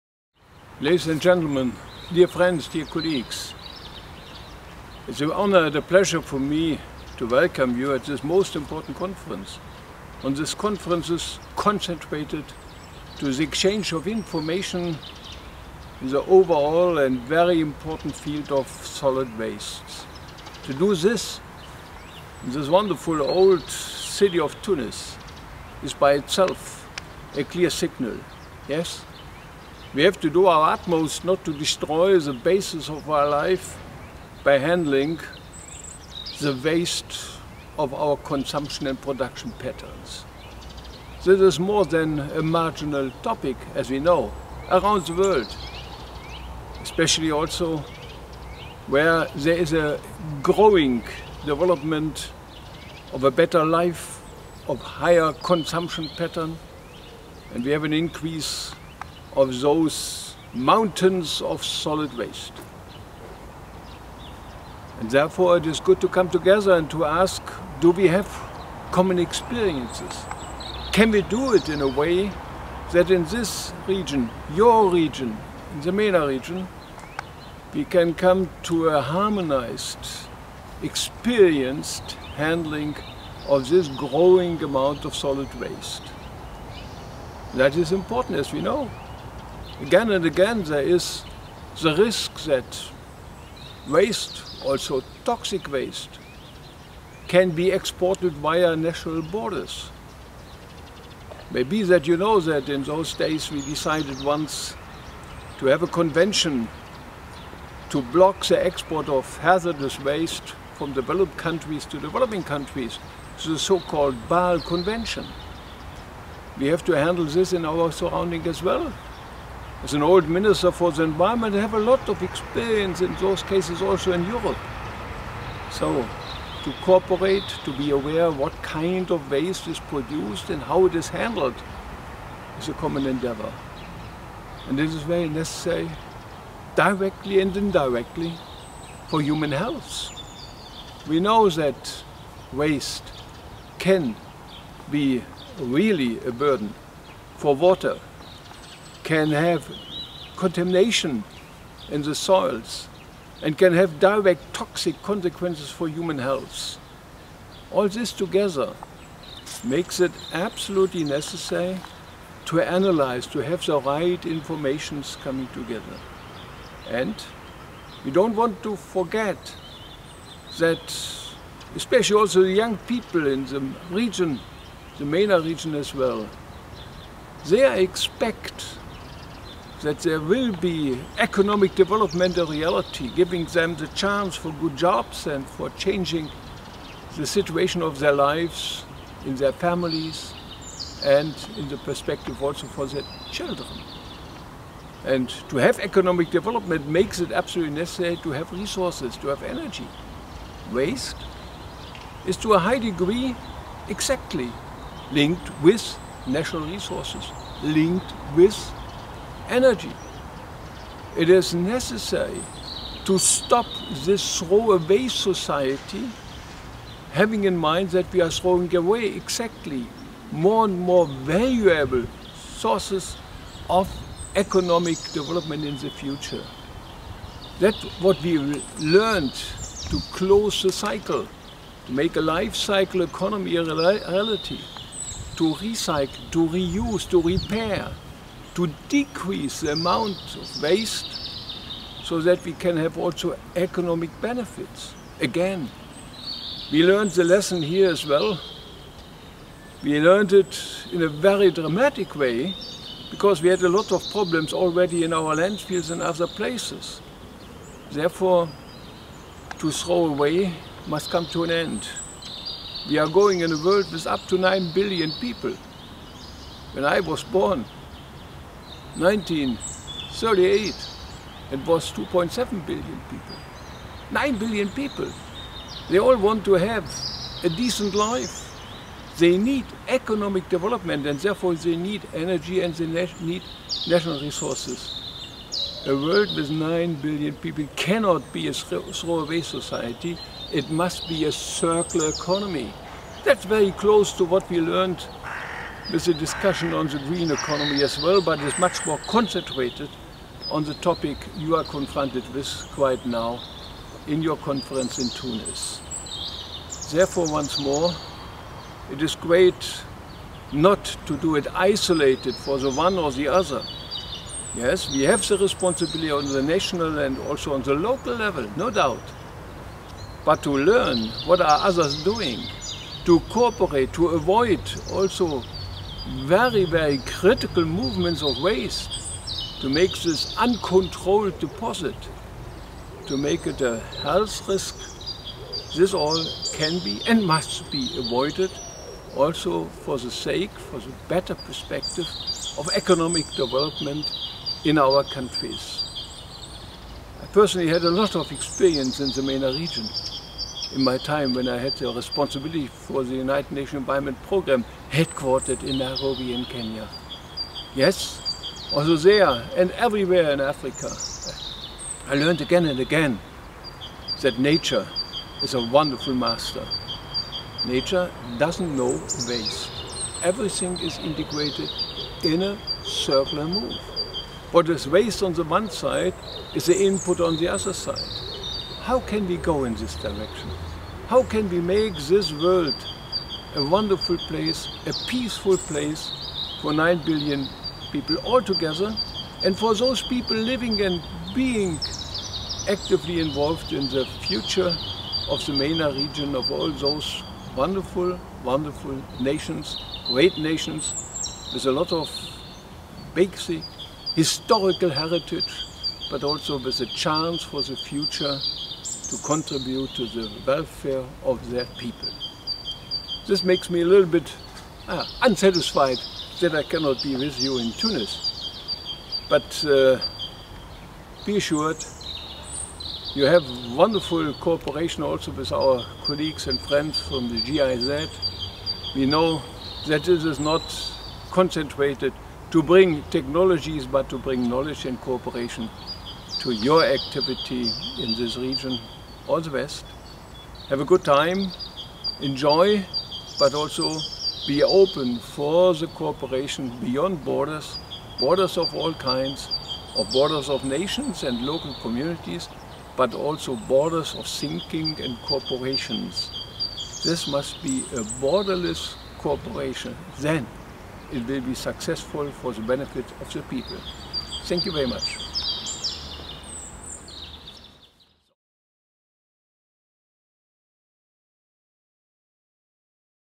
Klaus Topfer
Klaus Topfer's Keynote speach at the SWEEP-Net 5th Regional Forum on "Economic and Social Solutions for sustainable management of the environment" in Tunis/ Tunisiafrom April 14 to 16 2015.